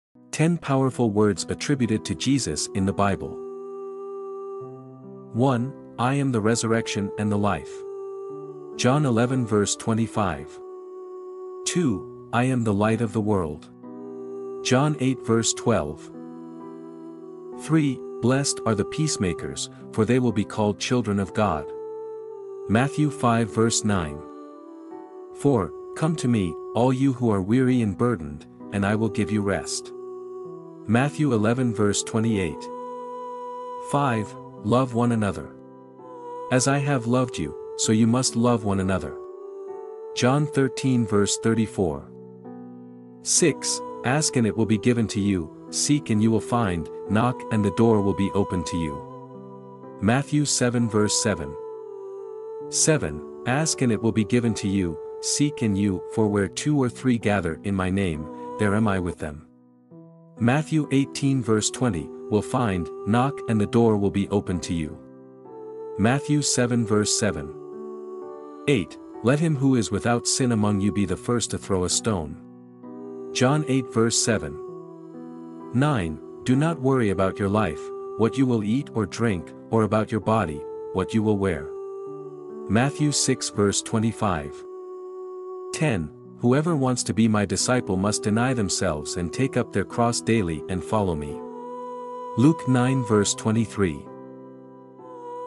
Eternal Wisdom: Jesus Christ's Timeless Teachings
Embark on a profound exploration of eternal wisdom as we delve into the timeless teachings of Jesus Christ. Discover the enduring relevance and transformative power of his messages, guiding us towards a deeper understanding of faith, compassion, and spiritual enlightenment.
Join us on a journey of reflection.